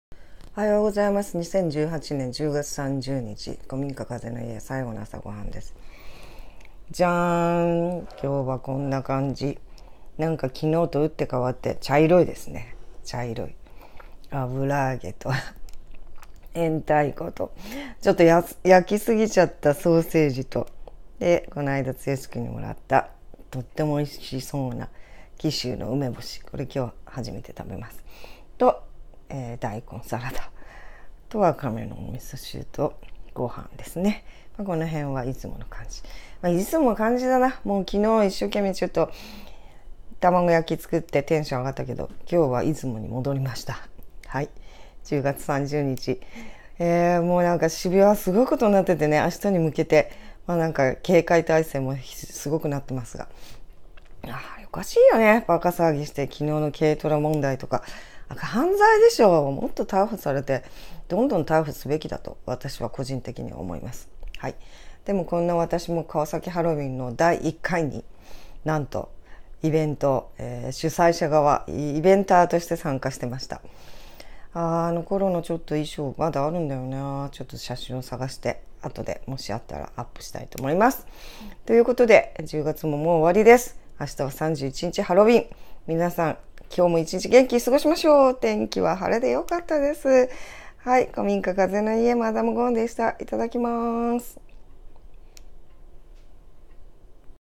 【最後の朝ごはん】油揚げ焼き、ソーセージ、焼き明太子、大根サラダ、梅干し、10月30日、古民家風の家
定番朝ごはんに戻りました